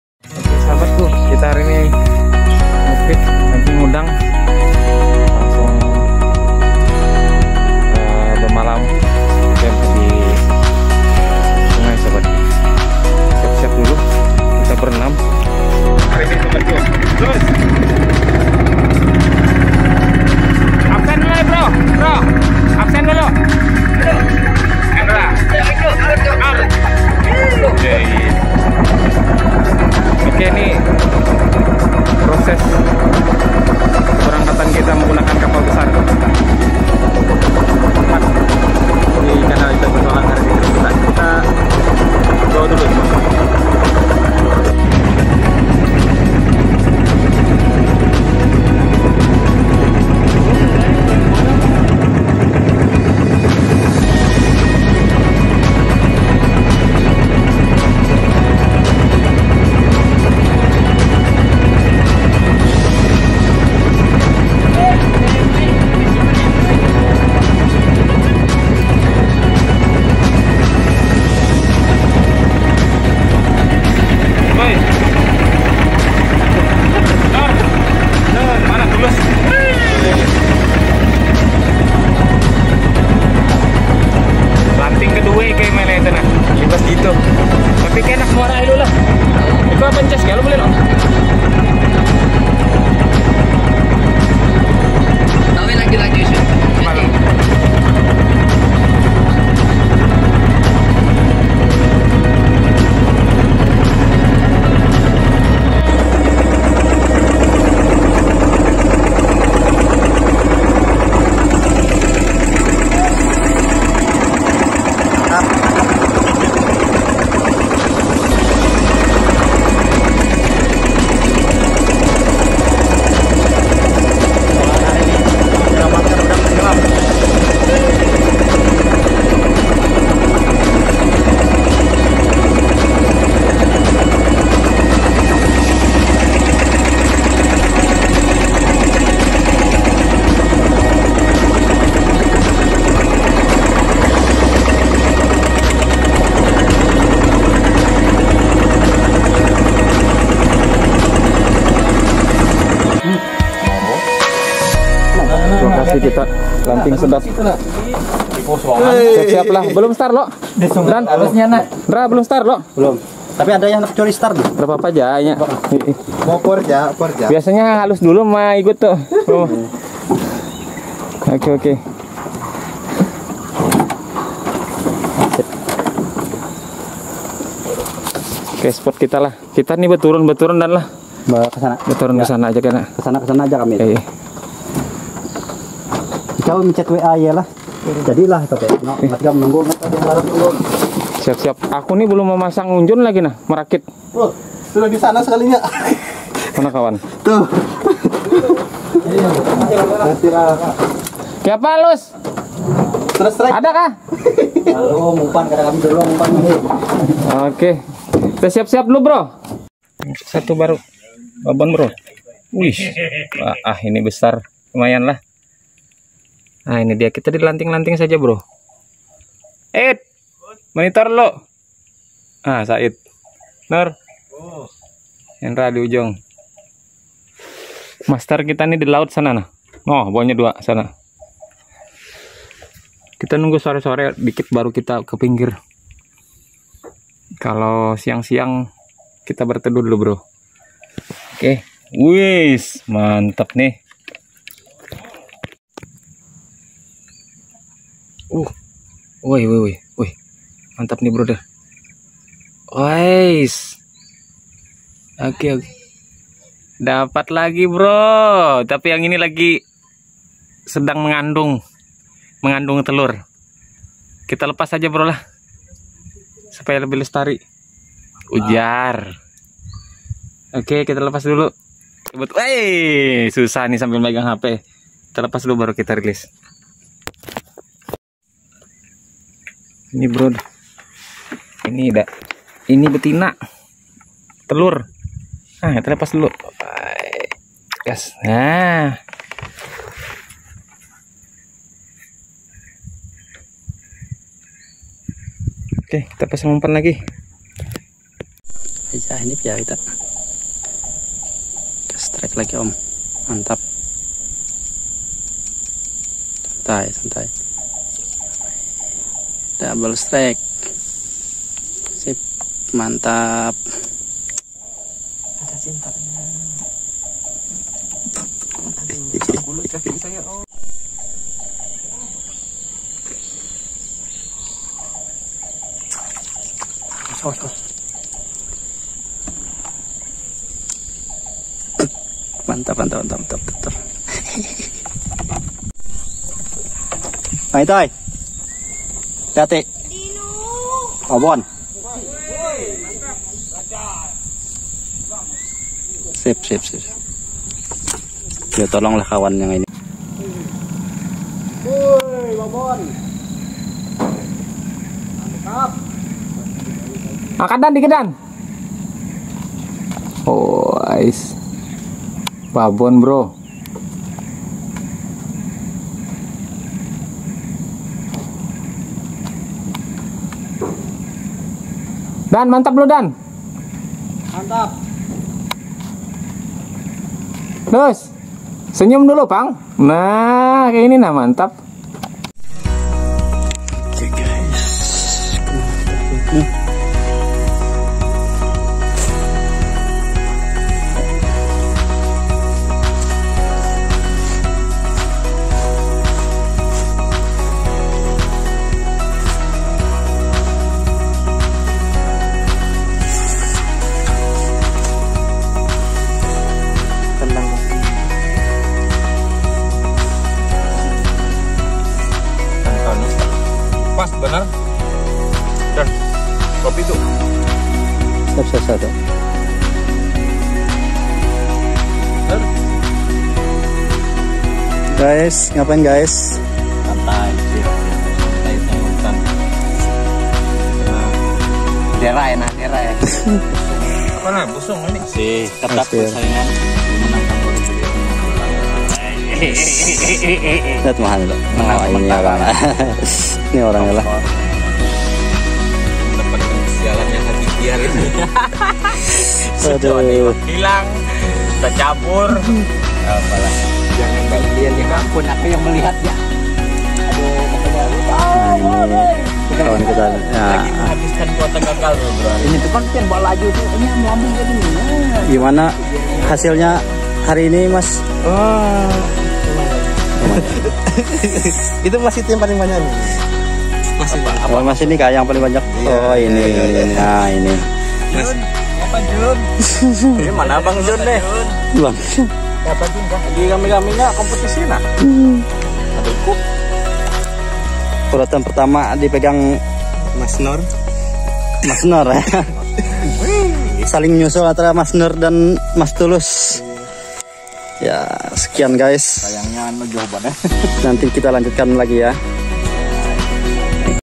"Petualangan Mancing Udang: Lepaskan Penat dan Nikmati Kebersamaan!"
Petualangan kita mancing udang galah bersama.sahabat kali ini seru seruan dan intinya kita cari hiburan bersama dan menambah pengalaman di dunia pemancingan udang, hasil nomor dua yang penting seru seruan dan melepas penat bersama para sahabat petualang